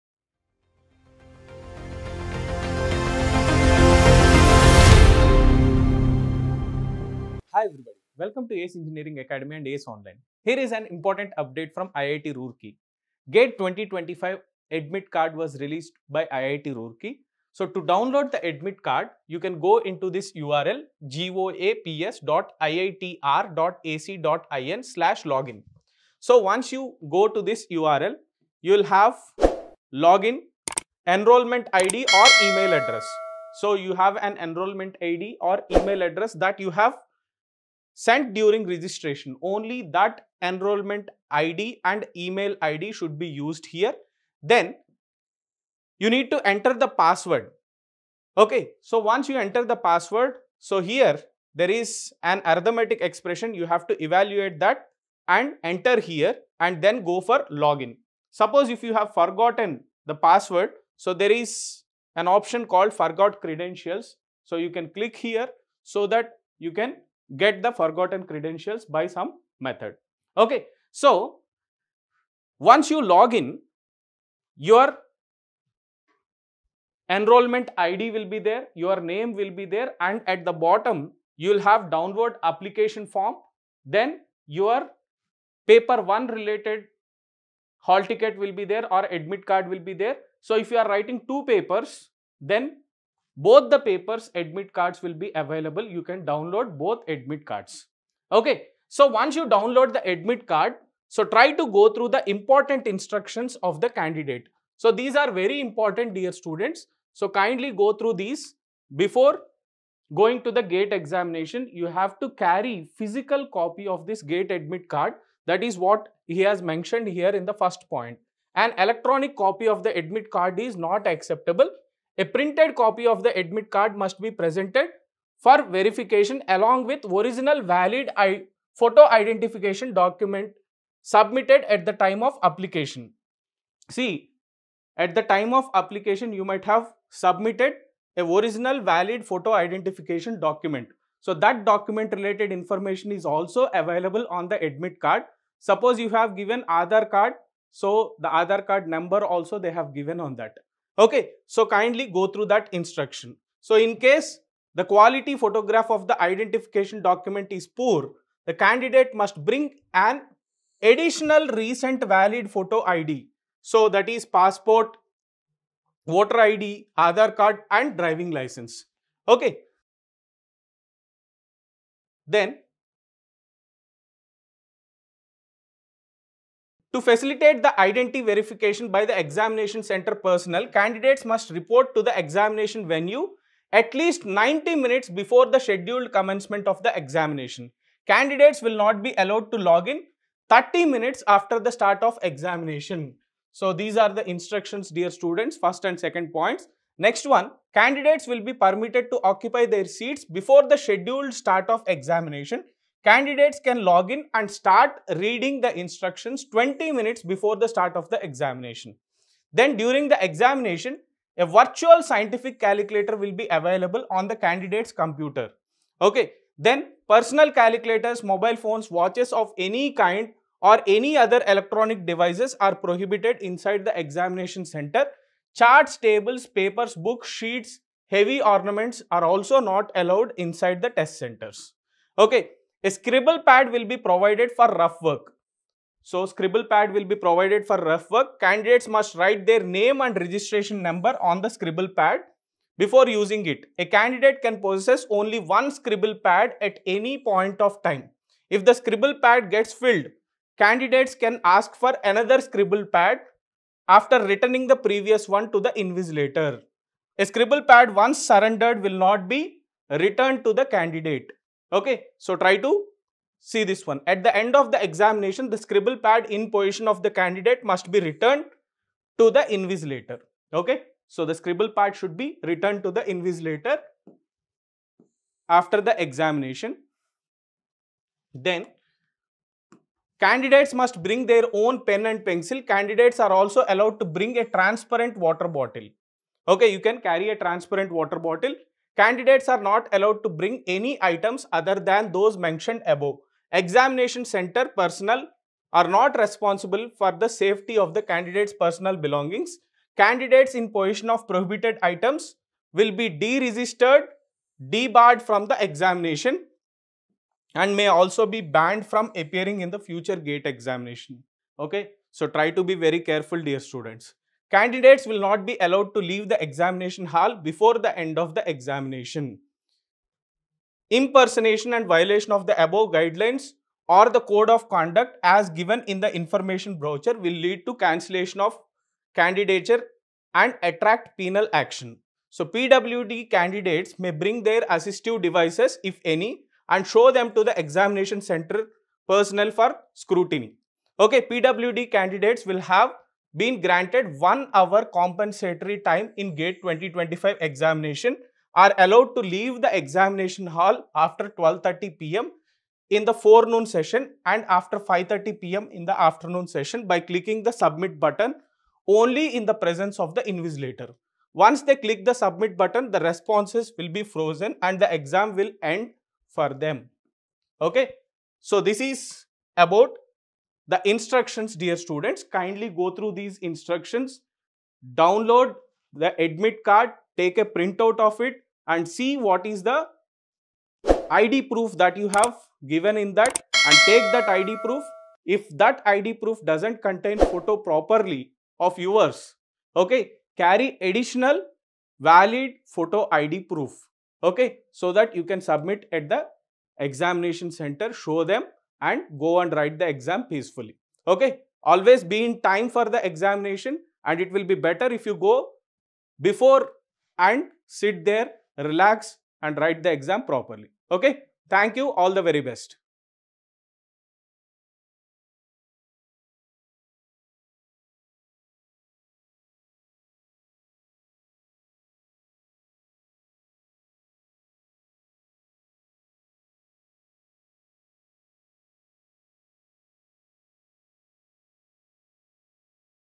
GATE 2025 Admit Card Released! | Download Process & Important Exam Instructions | ACE Online
The GATE 2025 Admit Card is officially released! 🎉 Learn how to download your admit card with our step-by-step guide and get important exam instructions to ensure a smooth exam day experience. Stay updated with all the essential details and tips for GATE 2025 preparation. Don’t miss out on these crucial insights to ace your exam. Watch now and stay ahead with ACE Online! 🚀